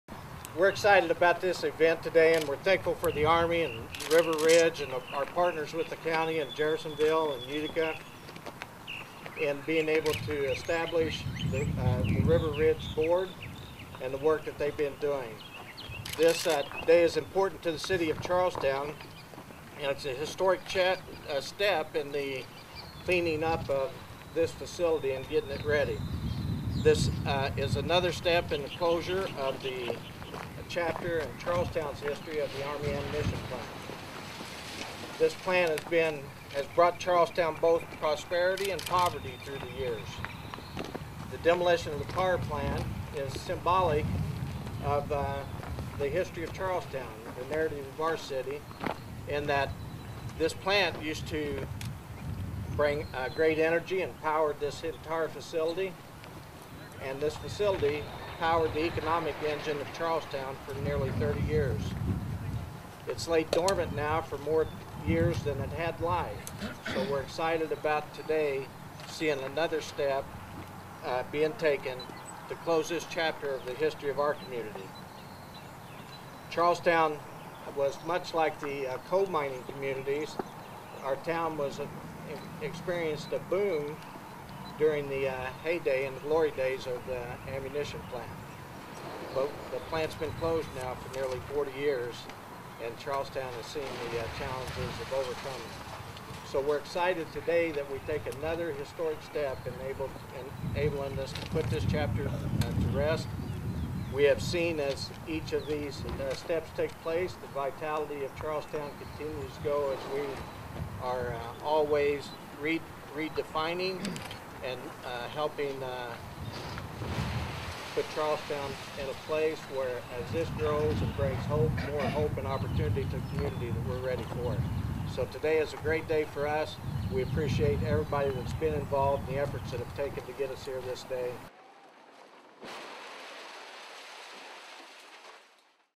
Charlestown Army Ammunition Plant Smokestack Demolition
This is short video of the demolition of a water tower and several smokestacks associated with a power plant near Gate 19 at the former Charlestown Army Ammunition Plant, now known as River Ridge Commerce Center. Charlestown's Mayor Bob Hall spoke at the event on March 22, 2012.